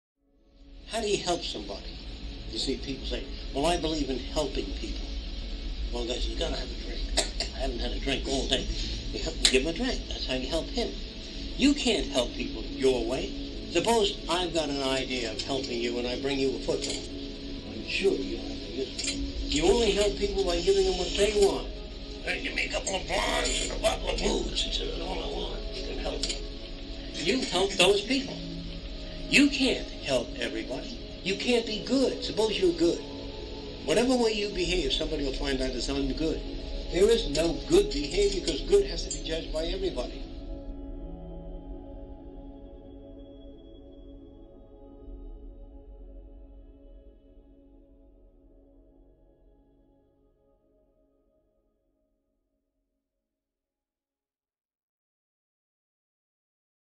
Jacque Fresco - Cómo ayudar a la gente
¿Cómo ayudas a alguien? Ves a la gente decir: "Bueno, yo creo en ayudar a la gente". Un tipo dice: "Tengo que tomar algo. No he tomado nada en todo el día". Si le quieres ayudar, dale de beber. Así es como le ayudas. No puedes ayudar a la gente a tu manera. Supongamos que tengo una idea de cómo ayudarte y te traigo un balón de fútbol. Estoy seguro de que no te serviría para nada. Solo ayudas a la gente dándoles lo que quieren. "Oye, dame un par de rubias y una botella de alcohol", dice. "Eso es todo lo que quiero si puedes ayudarme". Y tú ayudas a esas personas. No puedes ayudar a todos. No puedes ser bueno. Supongamos que eres bueno. Da igual cómo te comportes, a alguien siempre le parecerá que no está bien. No existe el “buen” comportamiento, porque tendría que ser juzgado por todos como “bueno”.

How do you help somebody? You see people say, "Well I believe in helping people." A guy says, "Got to have a drink. I haven't had a drink all day." You help him: give him a drink. That's how you help him. You can't help people your way. Suppose I've got an idea of helping you and I bring you a football. I'm sure you don't have any use for it. You only help people by giving them what they want. "Hey, give a couple of blondes and a bottle of booze," he says. "That's all I want if you can help me." You help those people. You can't help everybody. You can't be good. Suppose you're good. Whatever way you behave somebody will find that it is un-good. There is no good behavior because 'good' has to be judged by everybody.